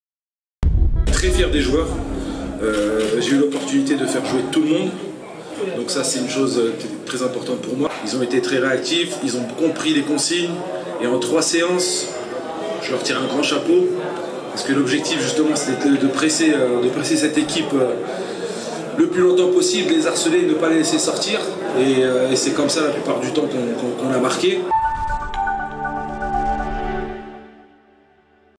الجزائر – ليبيريا: بوقرة يثني على أشباله في أول خرجة لهم
وهران (وأج)- أكد الناخب الوطني للاعبين المحليين مجيد بوقرة بأنه يؤمن بالمشروع الذي باشره مع هذا المنتخب، سيما بعد المردود الجيد الذي قدمه في أول مباراة ودية له أمام المنتخب الليبيري الأول والتي انتهت بفوز عريض (5-1) ليلة الخميس بالملعب الأولمبي الجديد بوهران.